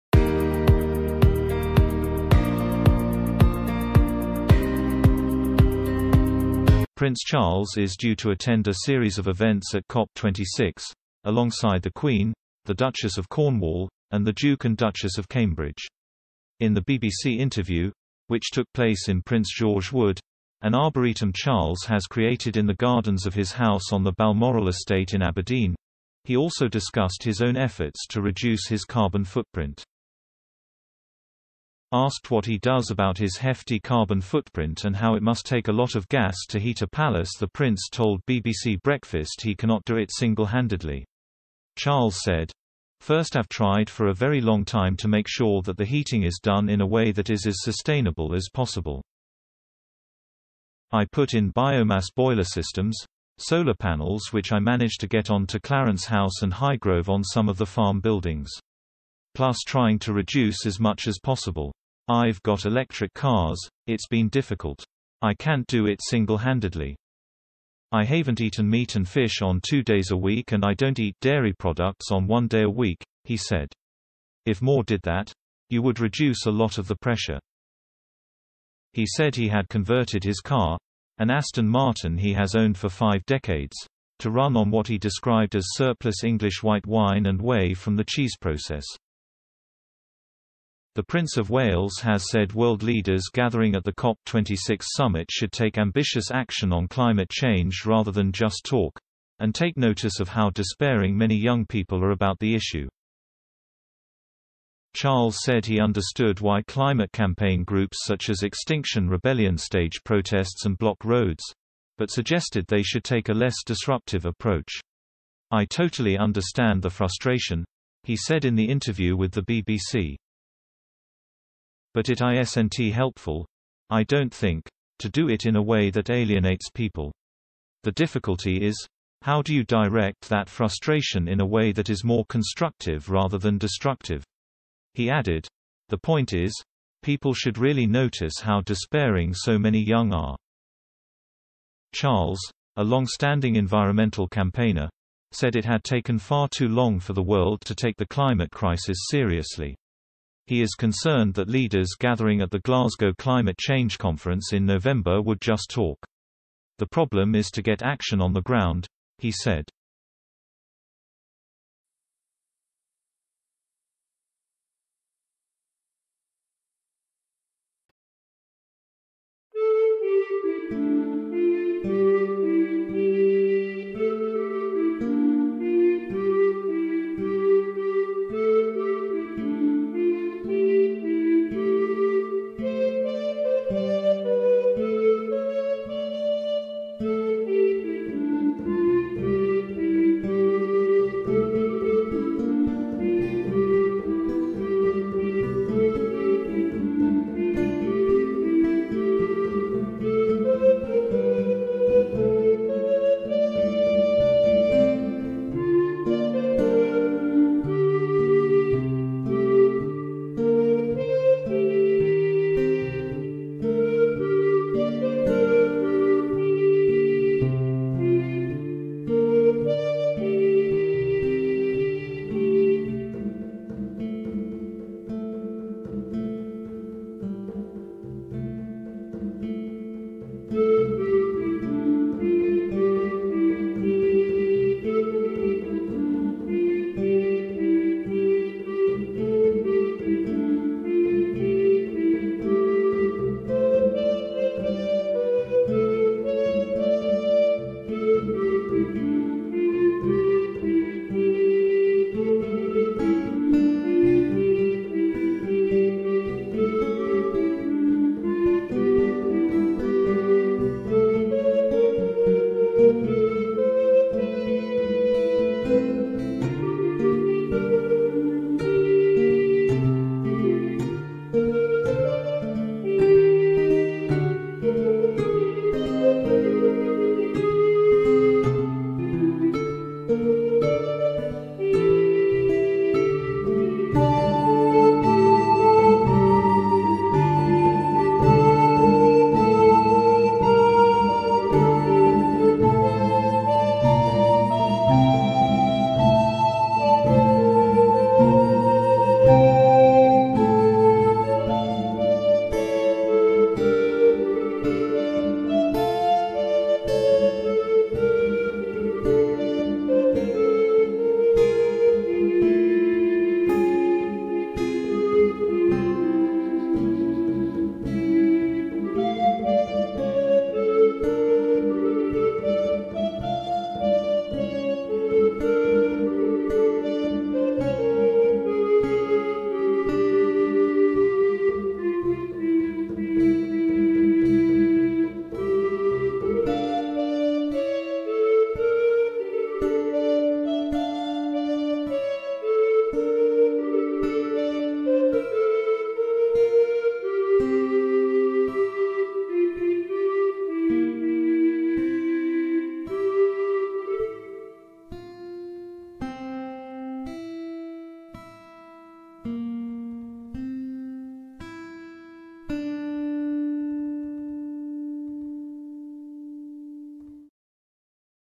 'Can't do it singlehandedly!' Prince Charles hits back as he's grilled on carbon footprint
In the BBC interview, which took place in Prince George's Wood, an arboretum Charles has created in the gardens of his house on the Balmoral estate in Aberdeen, he also discussed his own efforts to reduce his carbon footprint.

Asked what he does about his "hefty carbon footprint" and how it must "take a lot of gas to heat a palace" the Prince told BBC Breakfast he cannot "do it singlehandedly".

Charles said: "I have tried for a very long time to make sure that the heating is done in a way that is as sustainable as possible.

"I put in biomass boiler systems, solar panels which I managed to get on to Clarence House and Highgrove on some of the farm buildings.

"Plus trying to reduce as much as possible. I've got electric cars, it's been difficult. "I can't do it singlehandedly."

"I haven't eaten meat and fish on two days a week and I don't eat dairy products on one day a week," he said.

"If more did that, you would reduce a lot of the pressure."